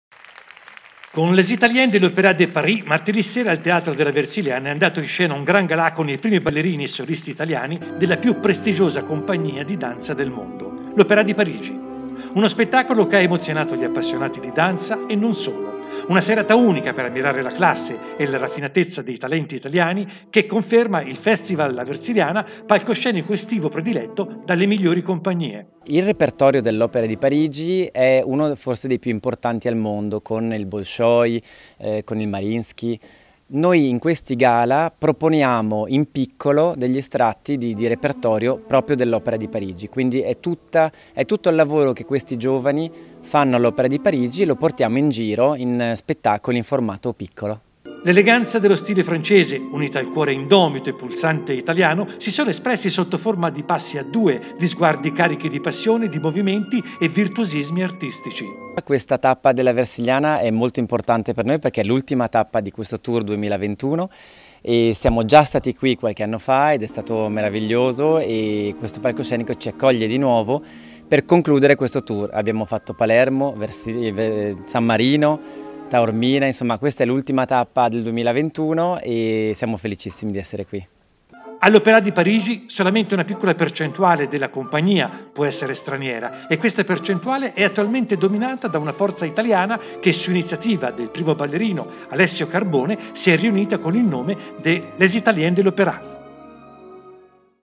La grande danza protagonista con "Les Italiens de l'Opera de Paris"
Con “Les Italiens de l'Opéra de Paris” martedi sera al teatro della Versiliana è andato in scena un gran gala con i primi ballerini e i solisti italiani della più prestigiosa compagnia di danza al mondo, l’Opéra di Parigi.